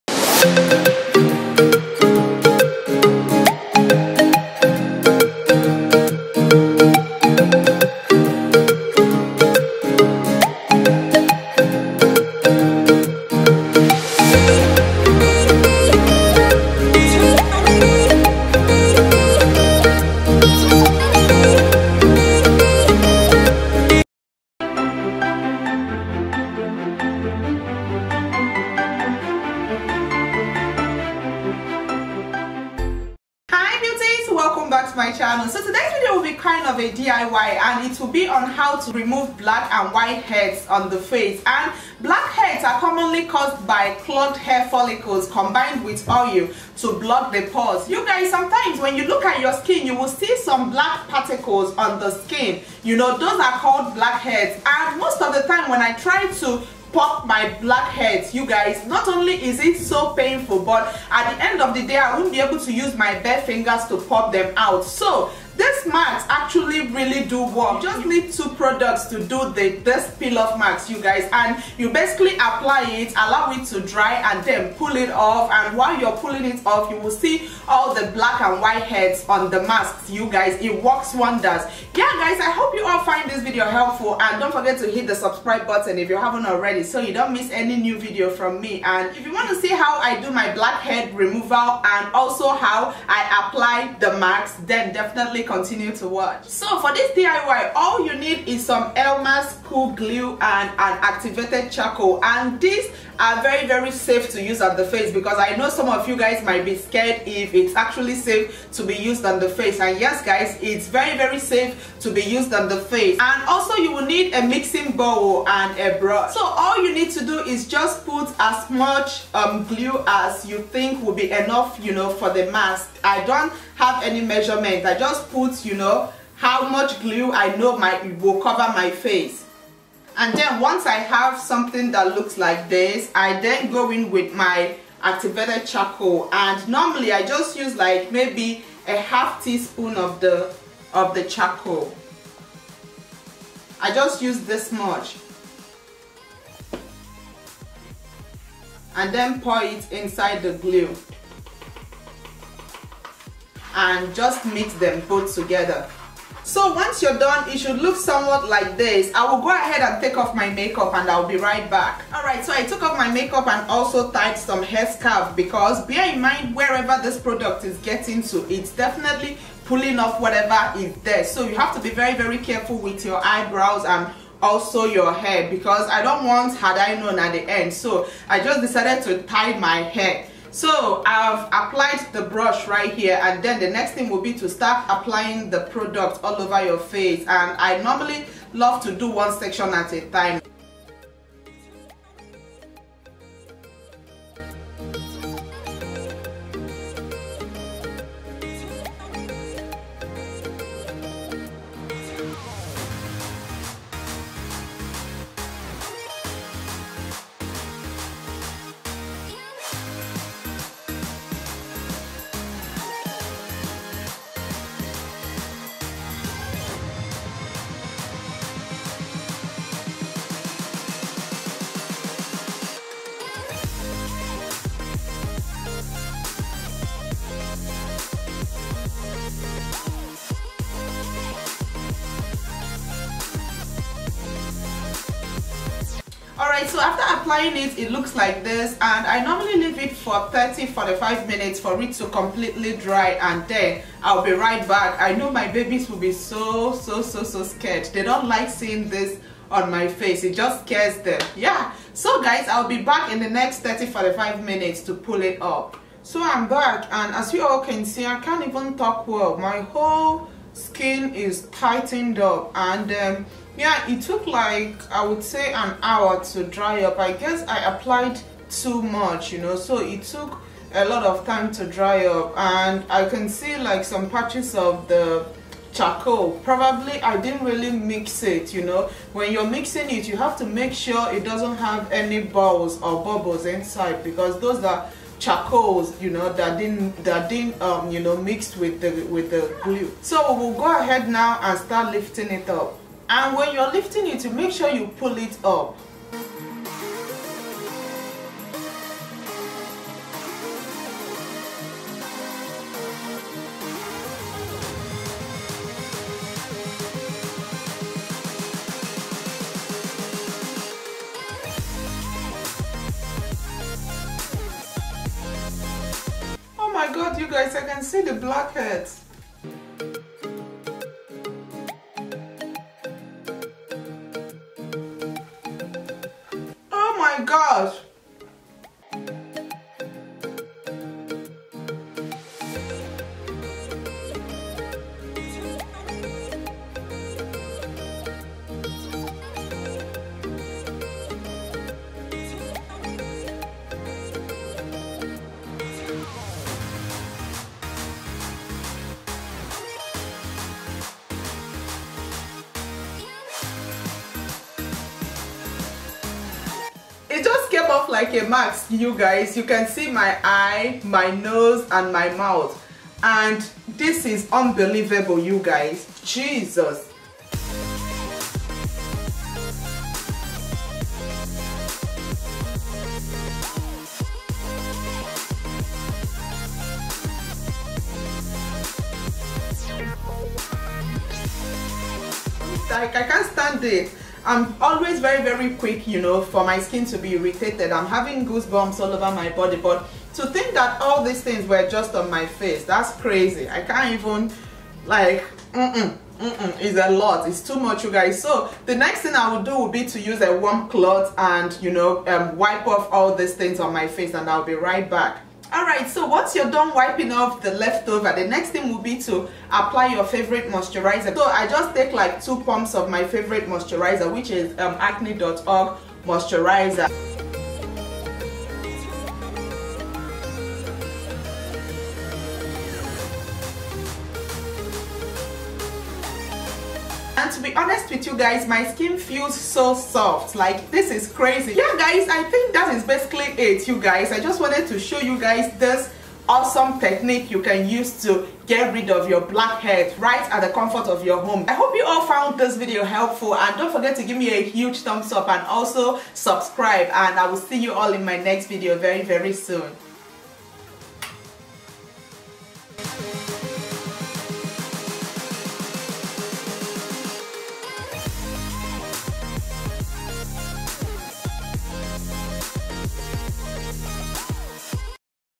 DIY BLACKHEAD REMOVER PEEL OFF MASK : HOW TO REMOVE WHITE & BLACKHEADS | OMABELLETV
WATCH MY FREE REPORT ON HOW I MAKE 6 FIGURES USING SOCIAL MEDIA AND MY PHONEs

Hey, Beauties! Here's a very effective and super easy at home diy blackhead remover peel off mask.
This technique is definitely worth giving a try as it removes every single blackhead out of the face.
Hope you all enjoy. xoxo!

The Glue I used

The Charcoal
Show less

Facebook.com/omabellebeauty --Like my page!!

Thank you so greatly for your support...